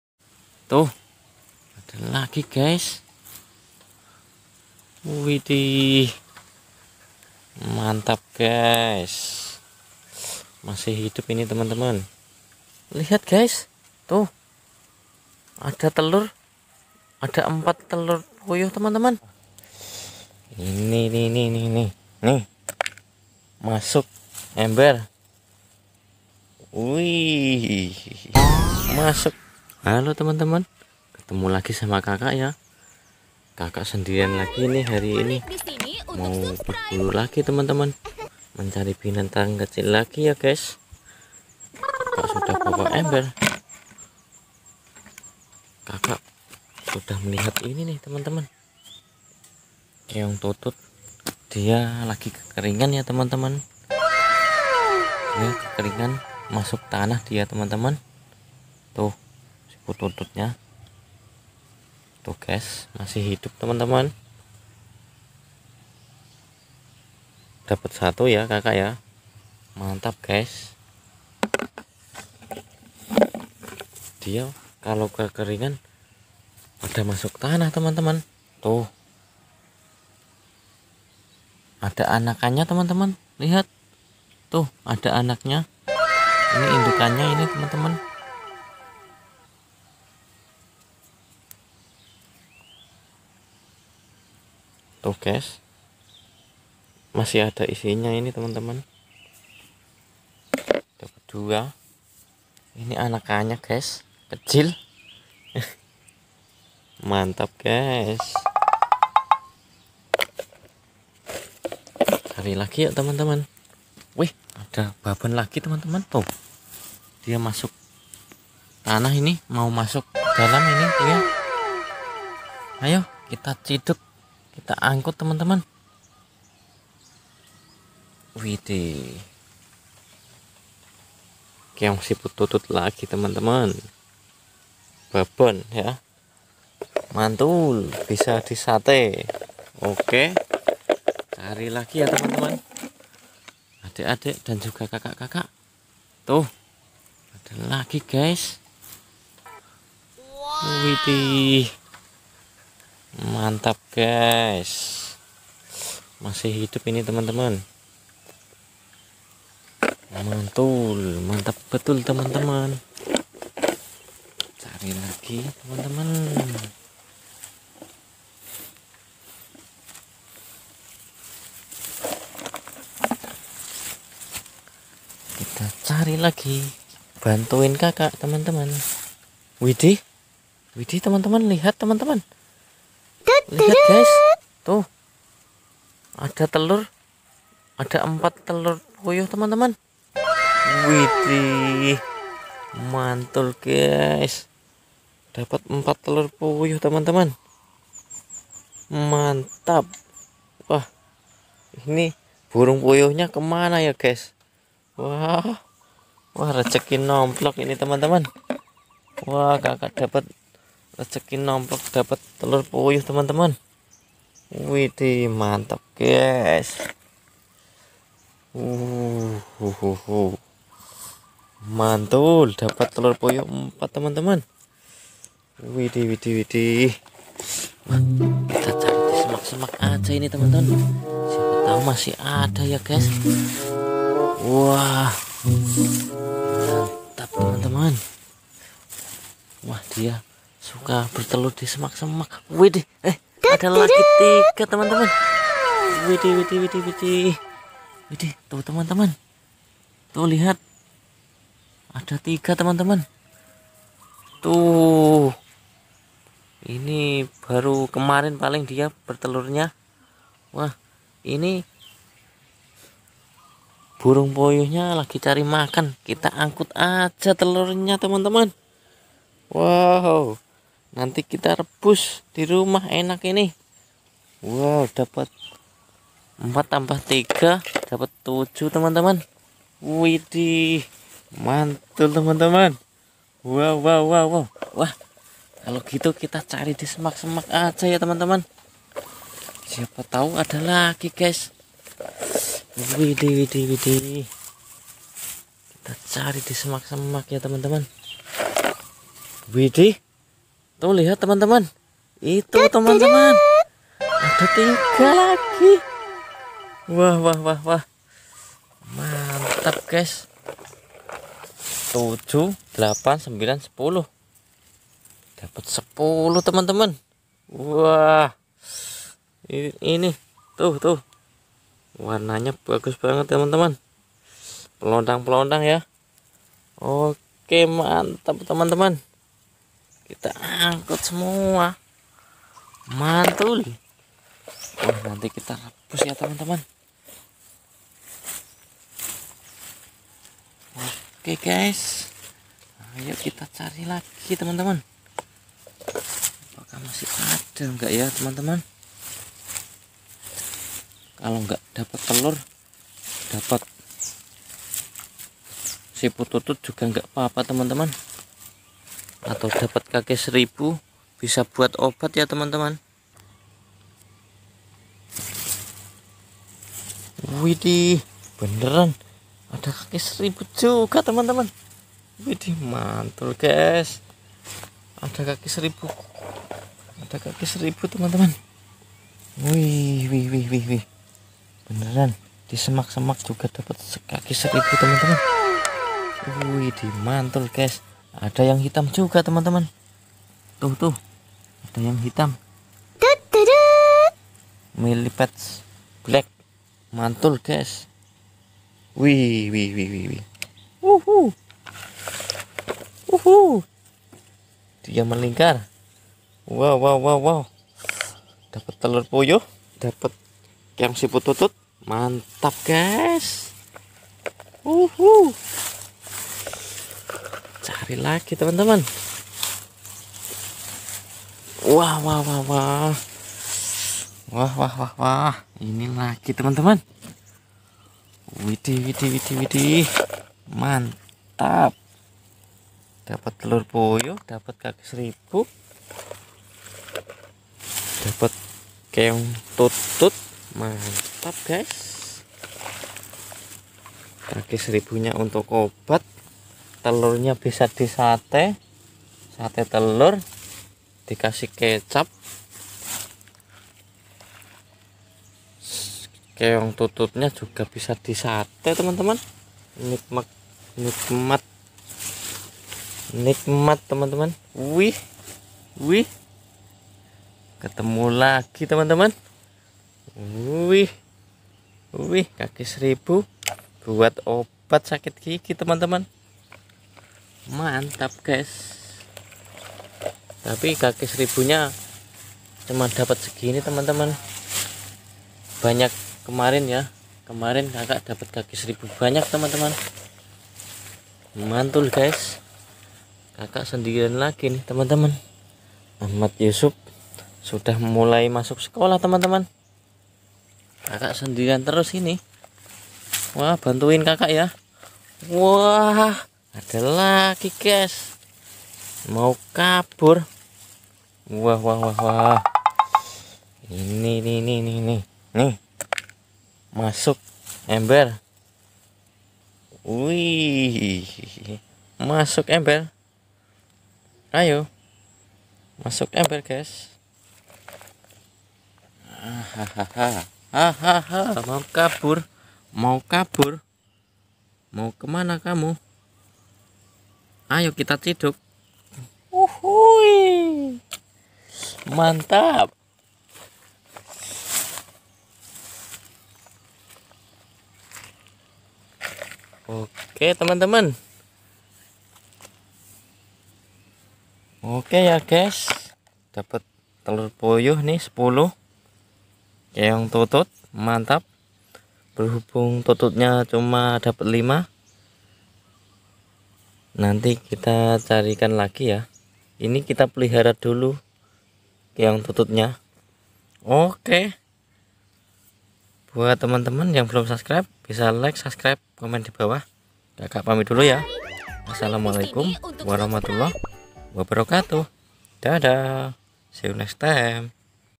menemukan siput tutut,telur puyuh,dan kaki seribu | tutut snail, quail eggs, millipede
mencari dan menemukan keong siput tutut,telur puyuh,luwing kaki seribu di semak semak
looking for and finding tutut snails, quail eggs, millipede luwing in the bushes